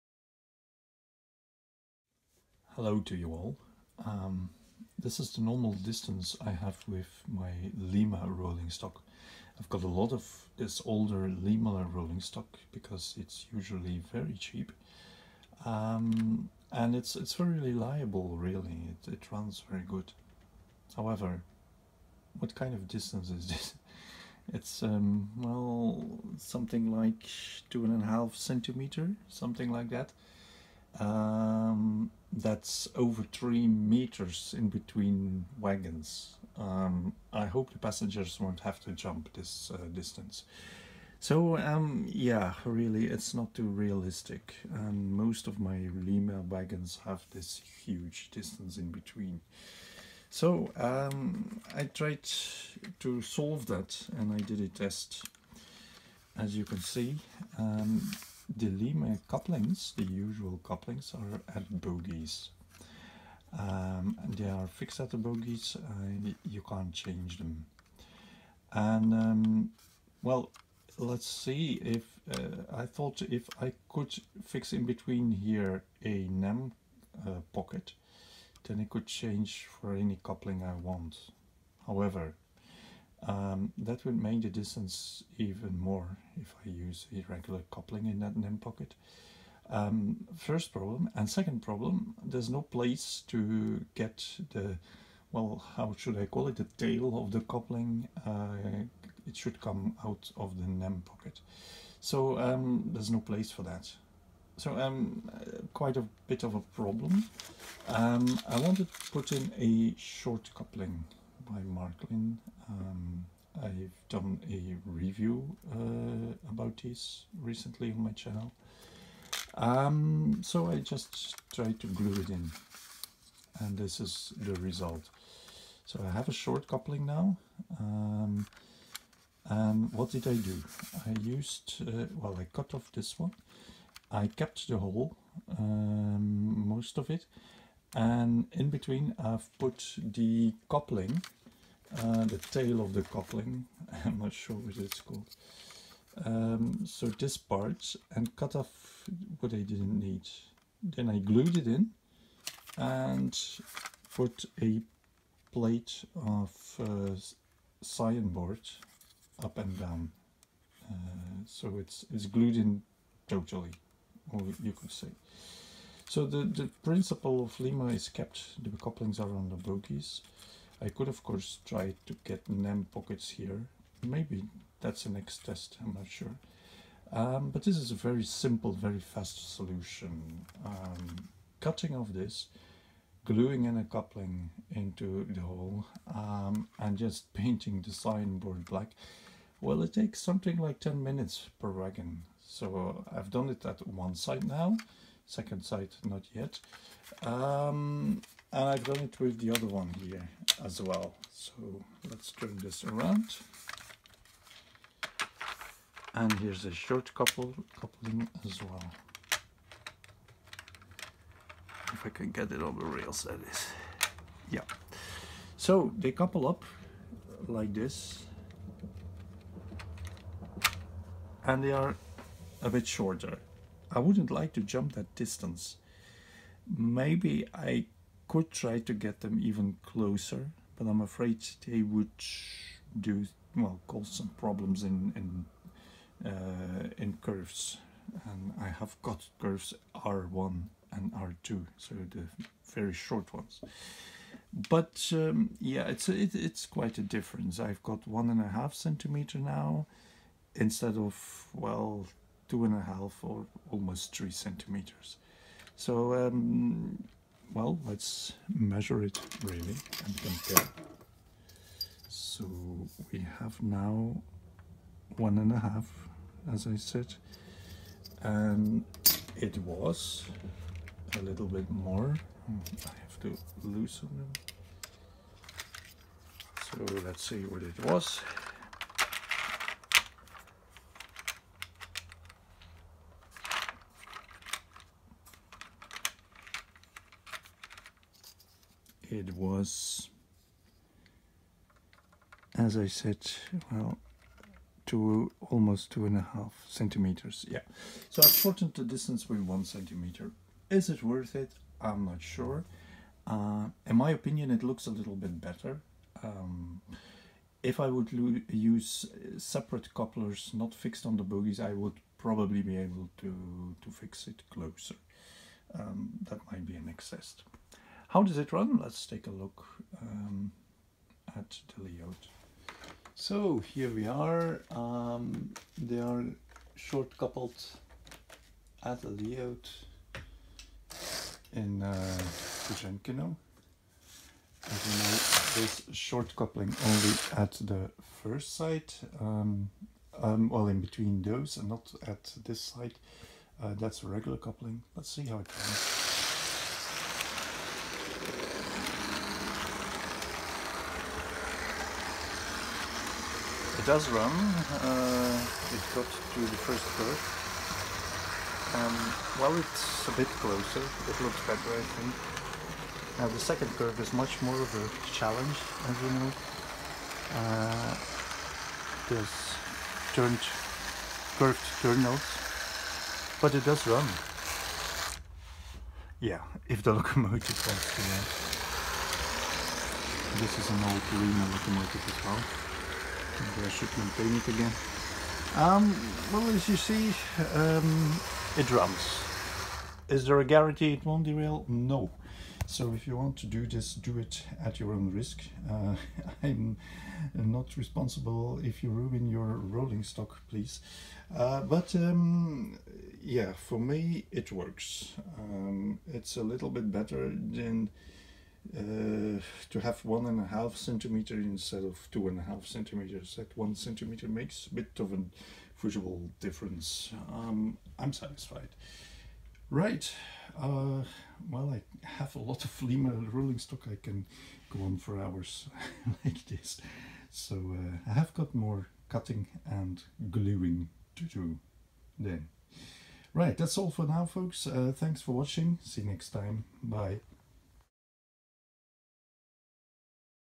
New Märklin 72000 Short couplers on old Lima rolling stock
It's probably not the best way to do this, sometimes wagons can derail (especially in short curves R1/R2), so no guarantees, do it at your own risk.
But for me it works, the distance between wagons is smaller and changing the couplers is easy and fast, albeit irreversible (you have to cut of the original Lima couplers).